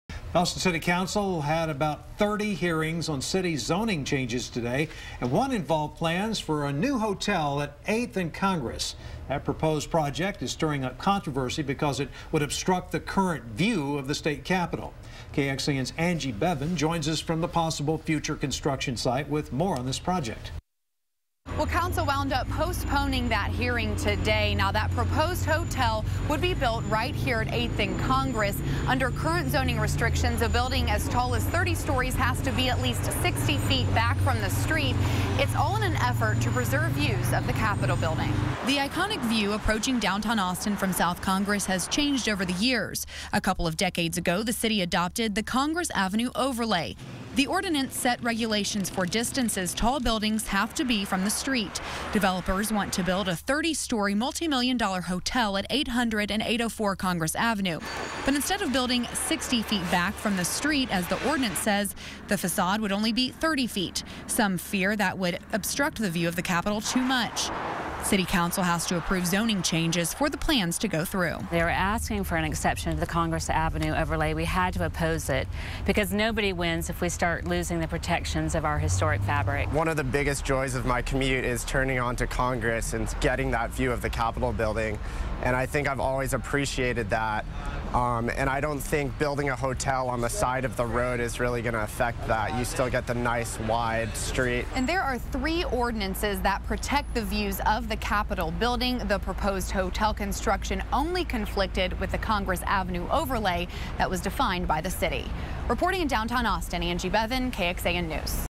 Planned hotel could obscure view of Capitol
The City Council is considering the high-rise at Eighth and Congress.